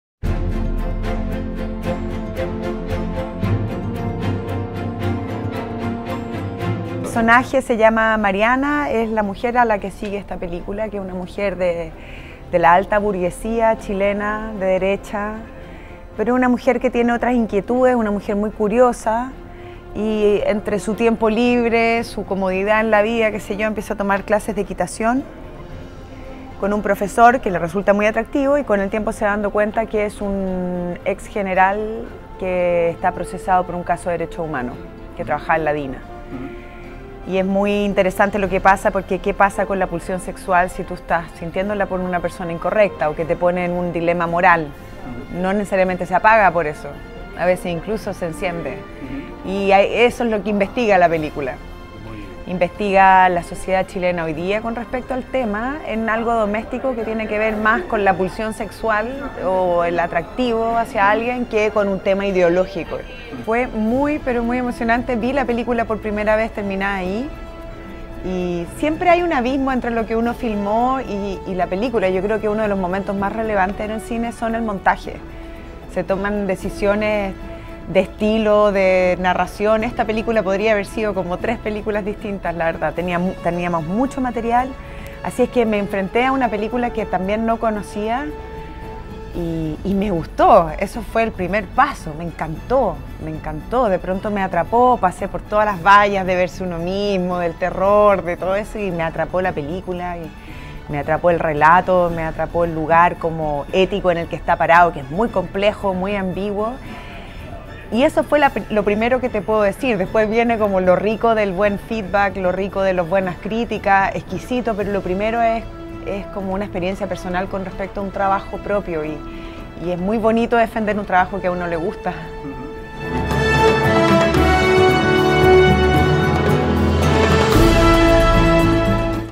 Premios PLATINO 2018 - Antonia Zegers presenta Los Perros
La actriz chilena Antonia Zegers, nominada al PLATINO a la Mejor Interpretación Femenina por El Club, presenta Los Perros, de Marcela Said. Este largometraje fue ovacionado en la pasada edición de la Semana de la Crítica de Cannes y participa en la quinta edición de los Premios PLATINO del Cine Iberoamericano, cuya gala de entrega se celebrará el 29 de abril en el Teatro Gran Tlachco de Xcaret (Riviera Maya, México).